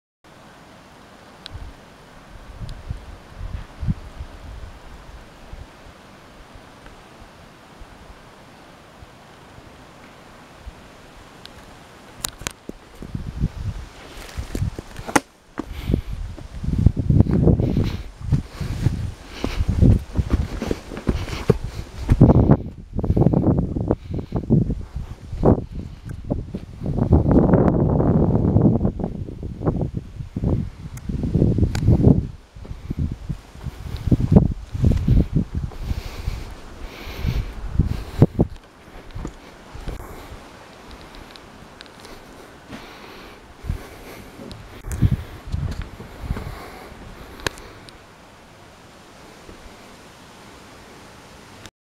Pillsbury Lake
Visiting the Pillsbury Lake Lean-To and sitting down by the lake for a few minutes in the West Canada Lake Wilderness.